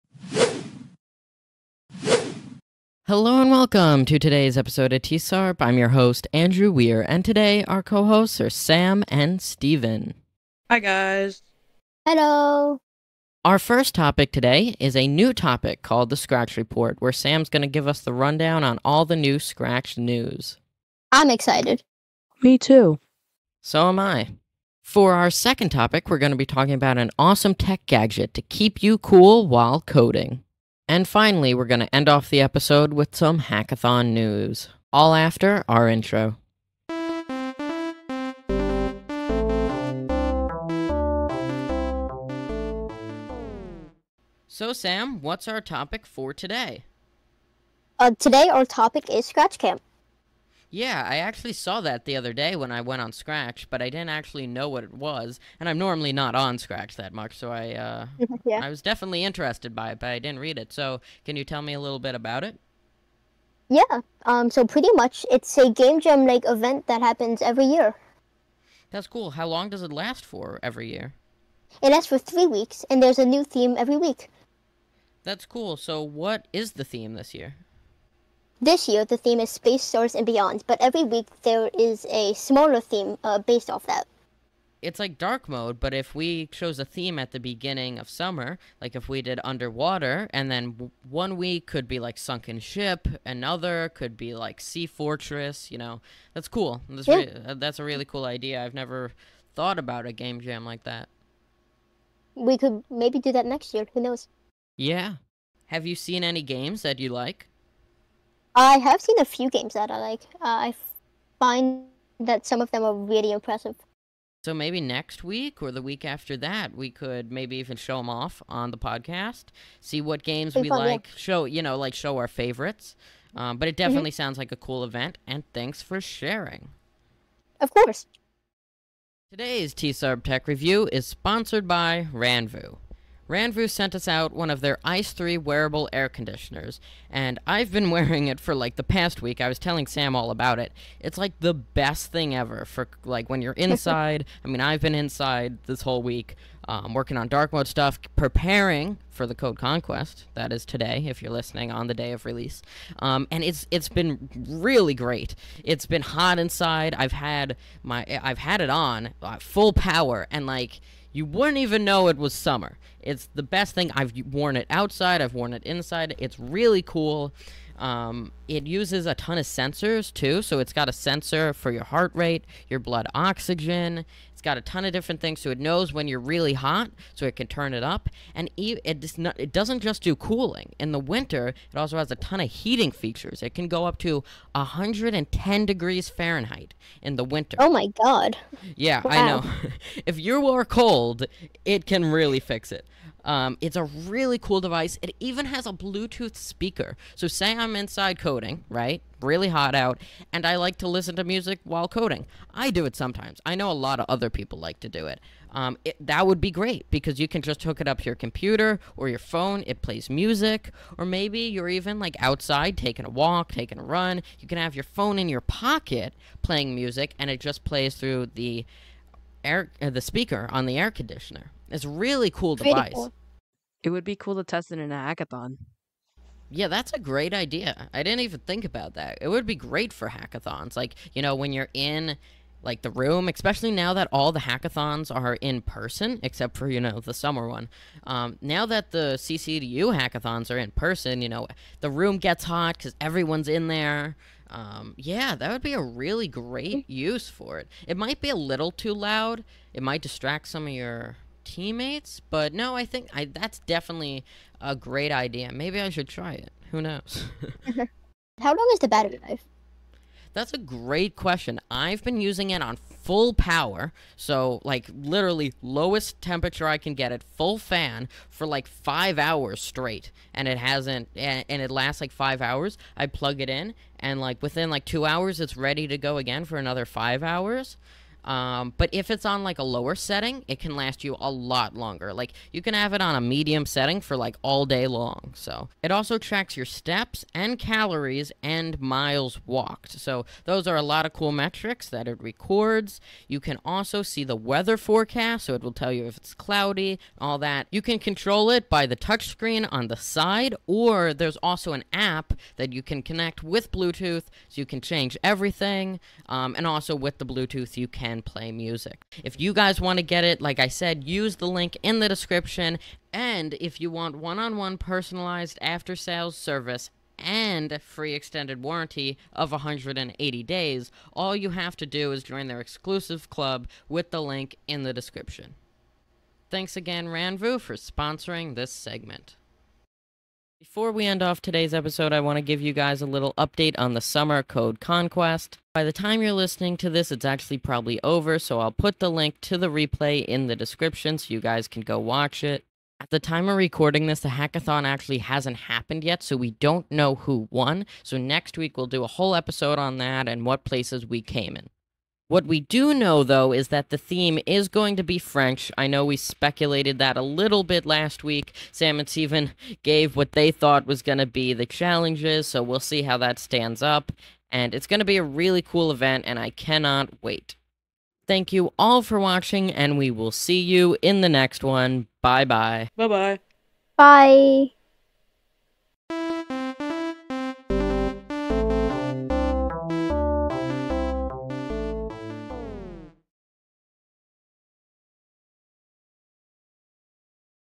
#156 of TSARP! Scratch Camp 2023 has started! Review of Ranvoo AICE3!
Today was the 156th episode!

Today's Topics:

- Scratch camp has started!

- Ranvoo AICE3 review!

Co-Hosts: Sam & Steven